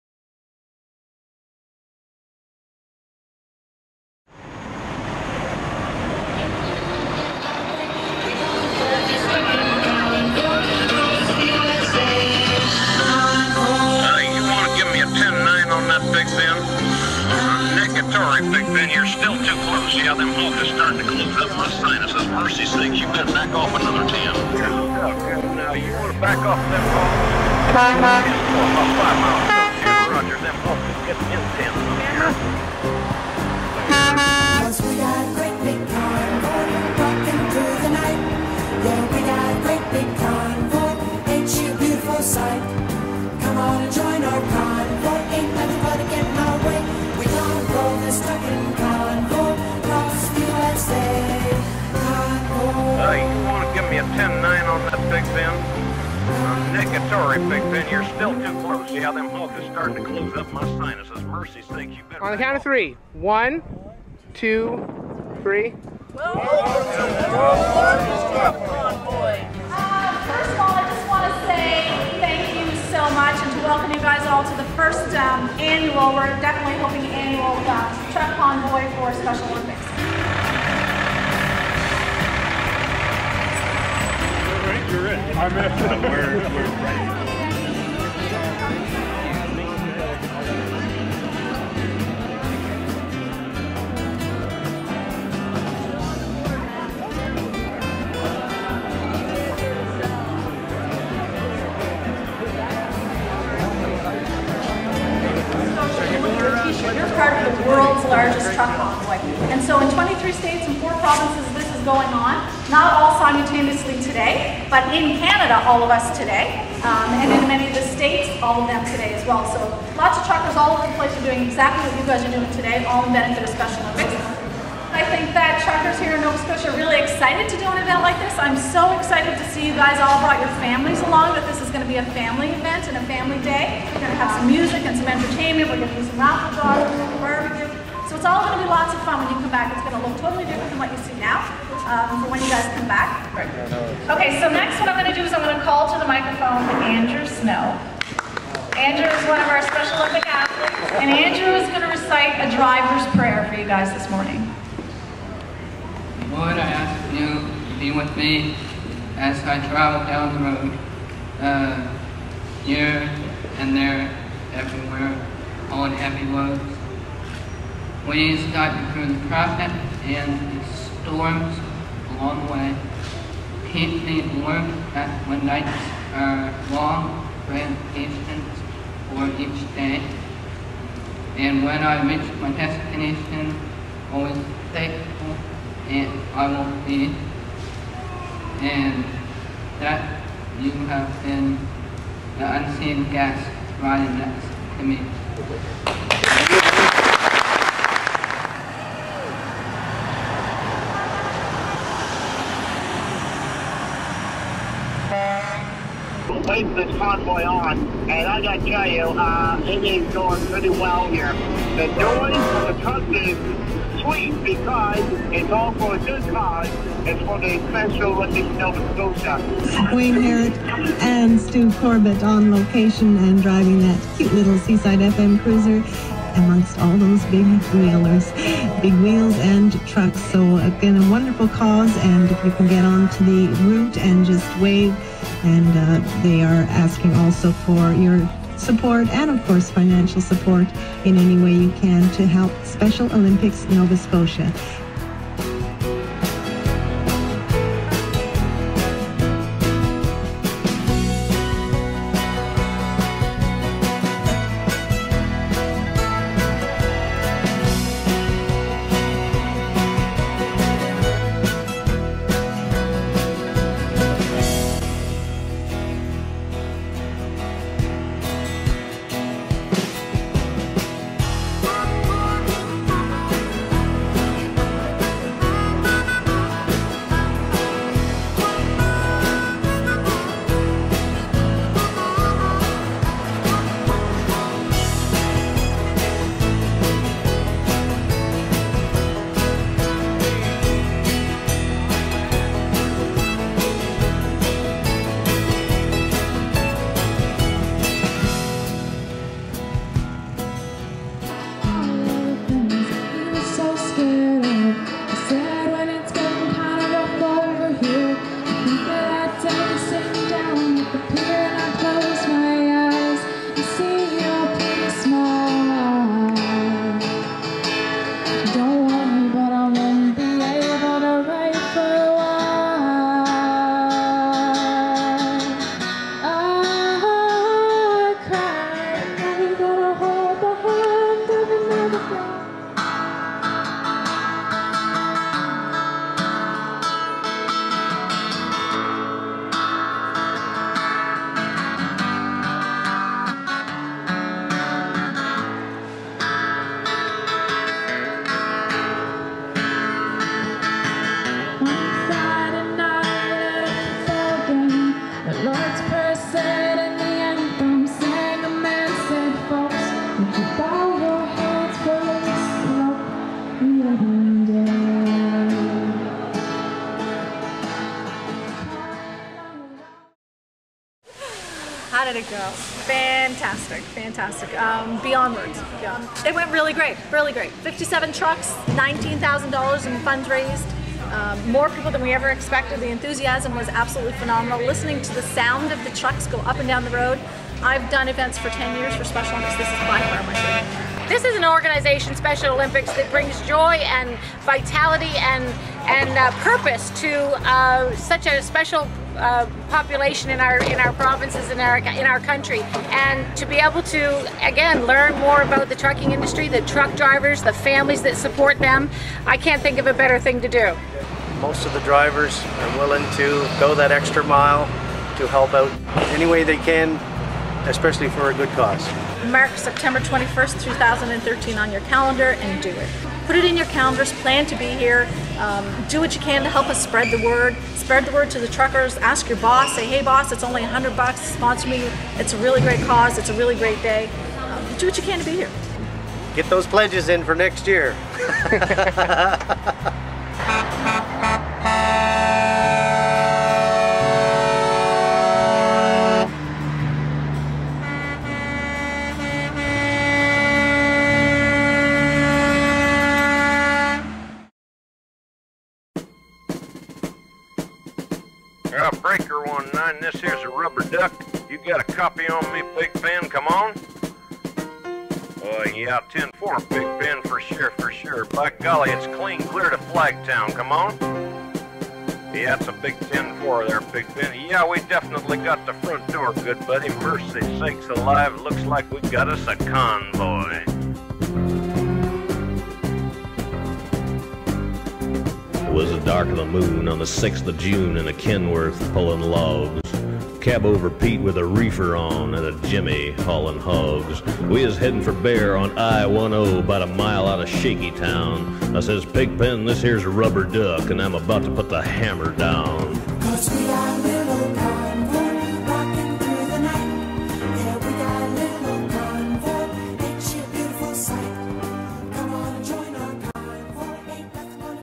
2012 Worlds Largest Truck Convoy for Special Olympics Nova Scotia
Official video of the 2012 World's Largest Truck Convoy for Special Olympics Nova Scotia. Video: Christopher Muise. Photos: Steve Auchu.